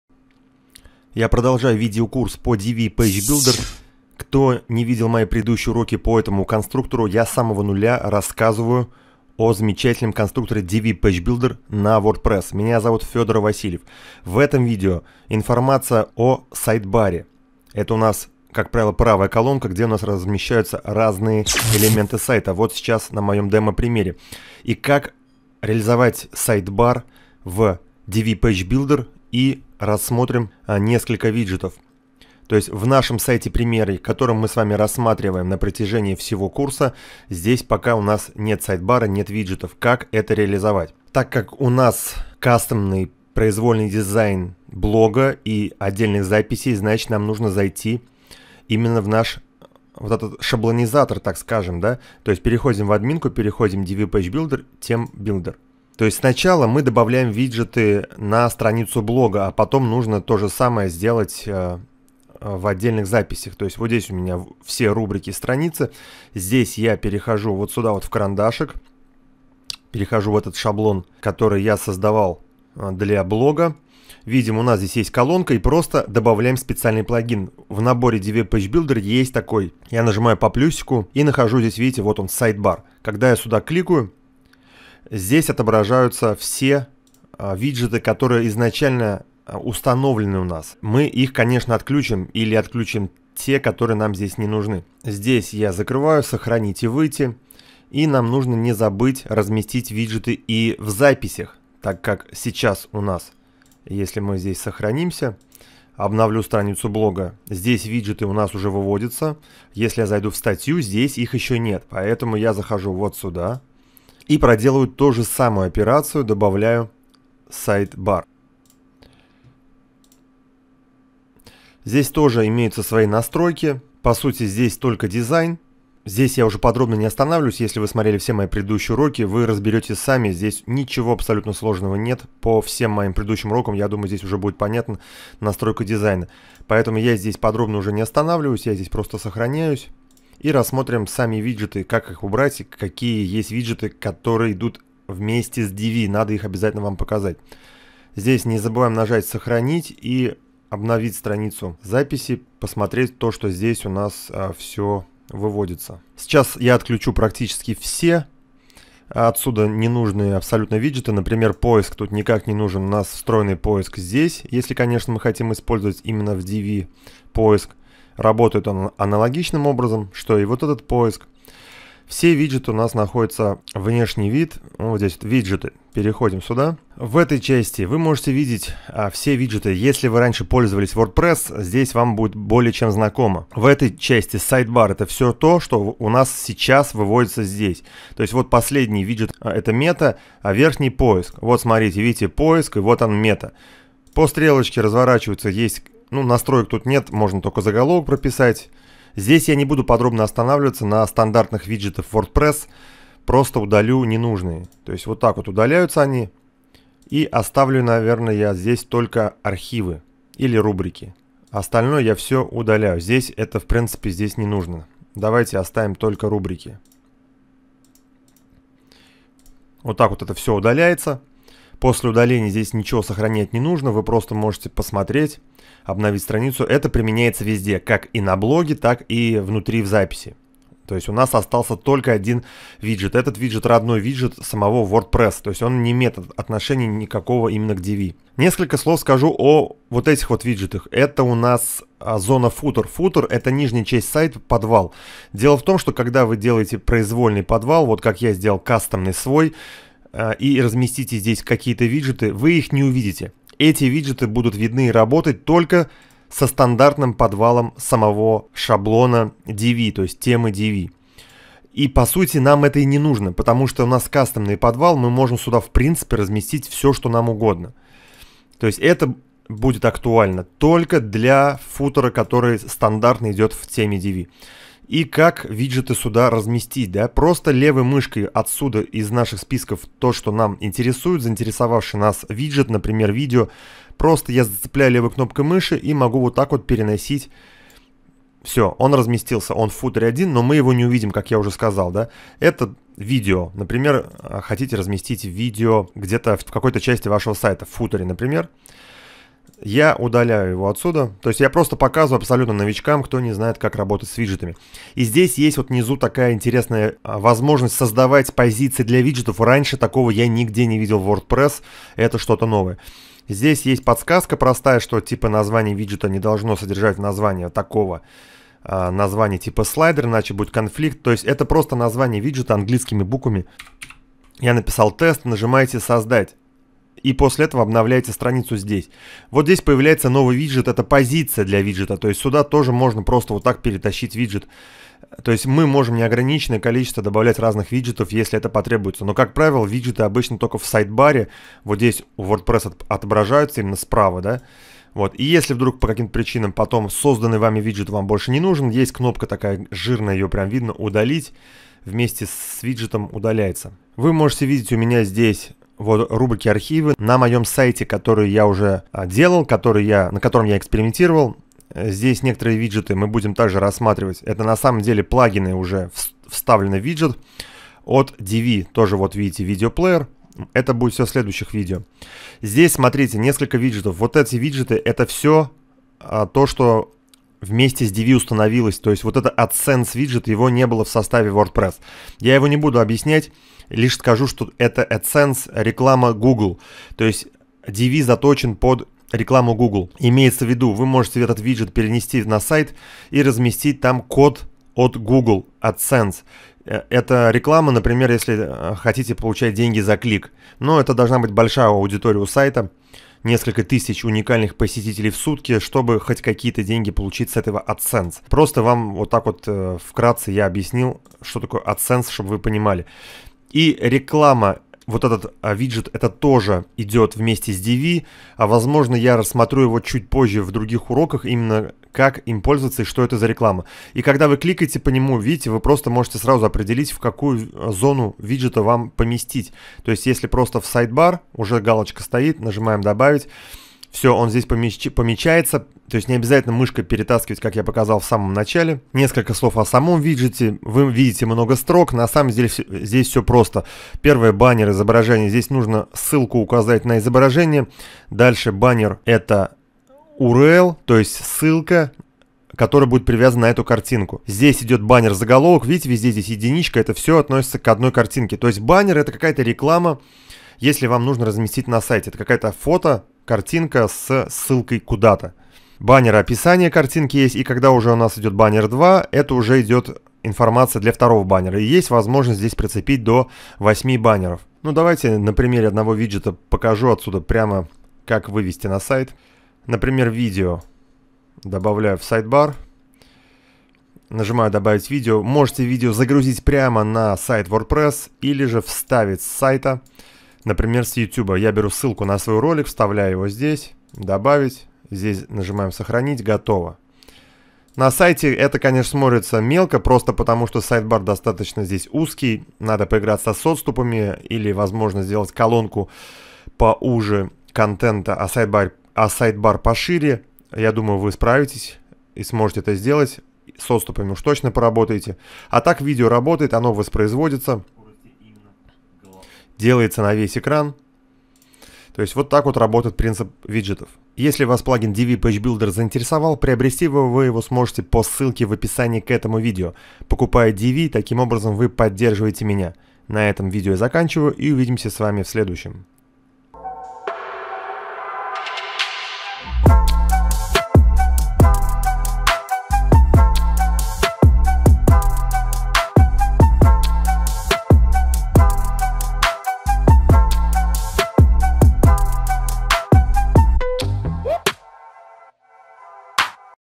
Настройки виджетов в Divi Page Builder #14
В этом видео выведем сайтбар на сайт в Divi Page Builder, а так же рассмотрим что такое виджеты и как их использовать.